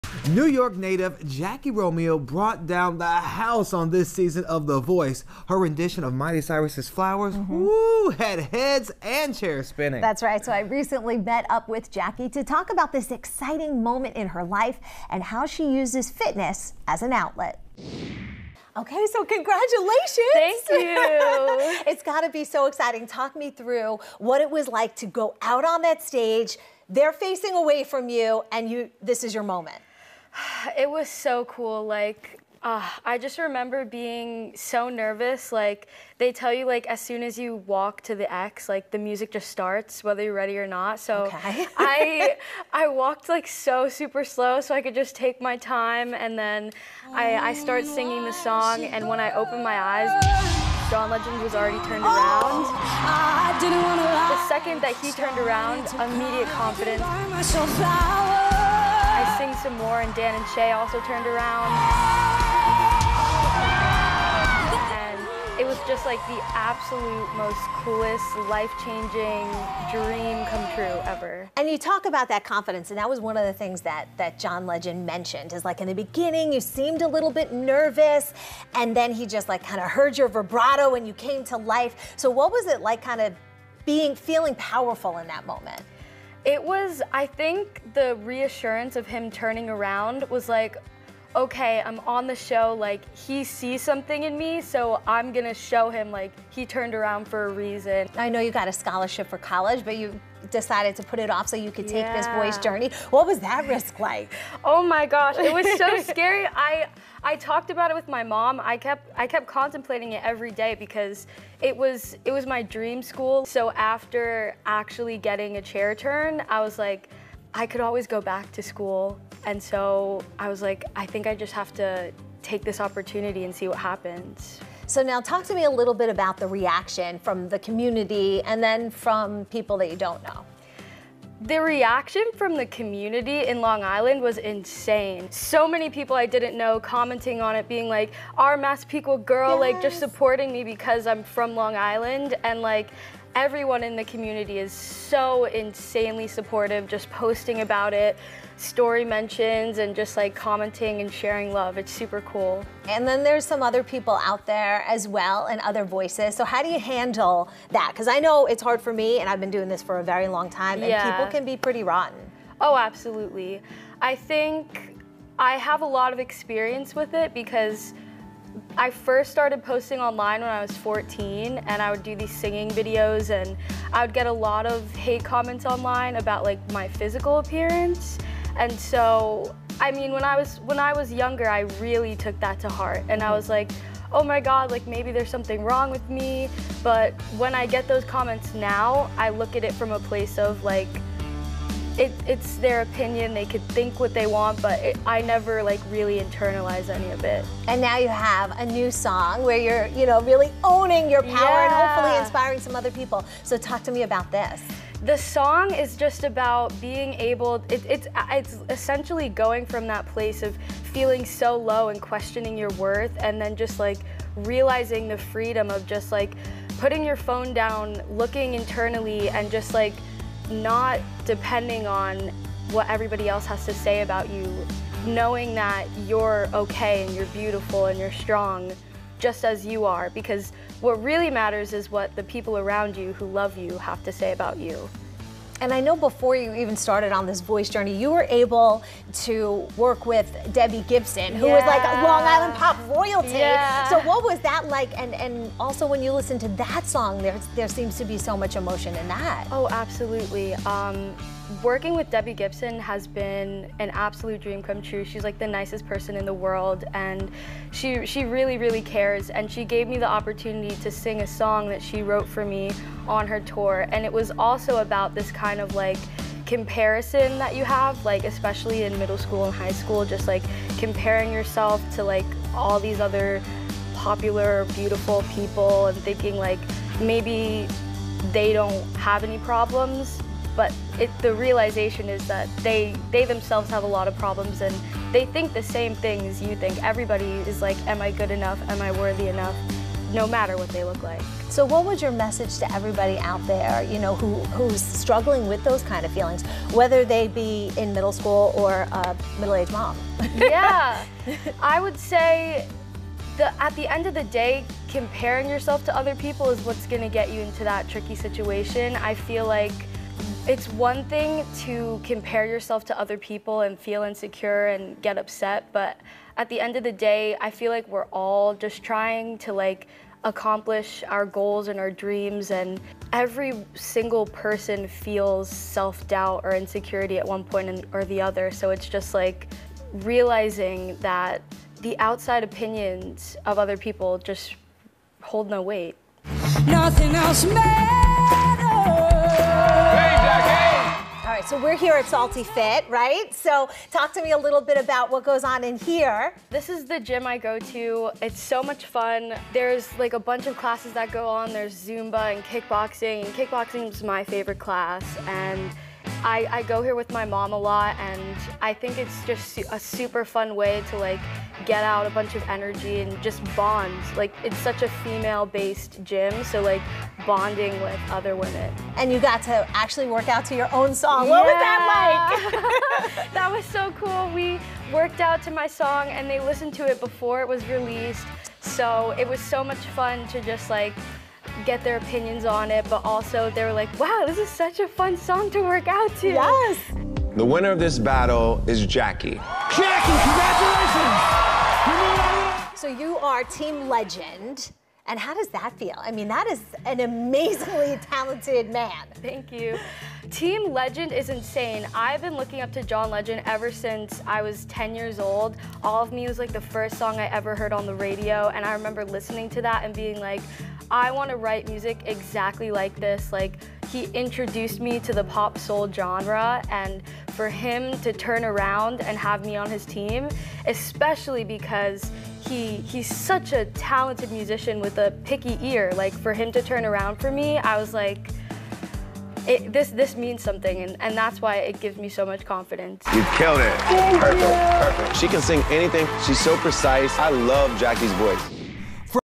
Jackie Romeo shares how competing on ‘The Voice’ changed her life | News 12
Singing sensation Jackie Romeo tells beWell how competing on “The Voice” changed her life and how exercise helps her stay mentally and physically healthy.